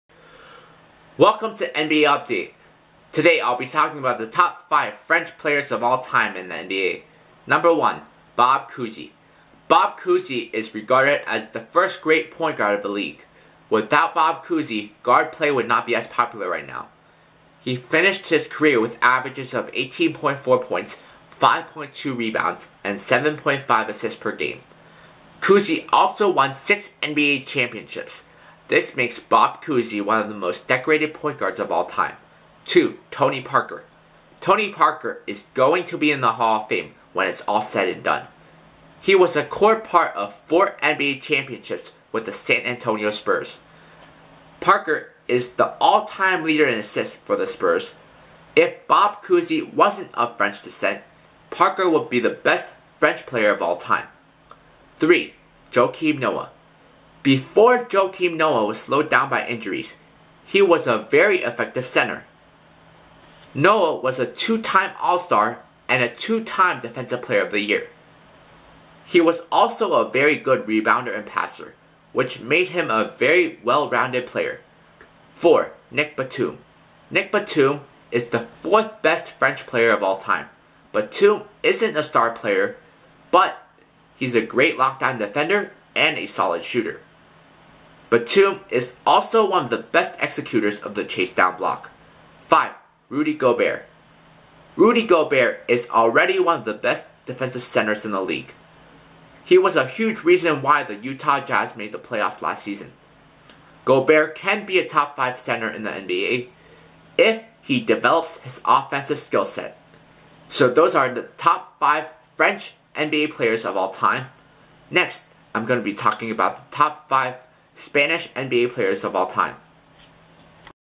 TOP 5 FRENCH NBA PLAYERS OF ALL-TIME
1. Bob Cousy

2. Tony Parker

3. Joakim Noah

4. Nic Batum

5. Rudy Gobert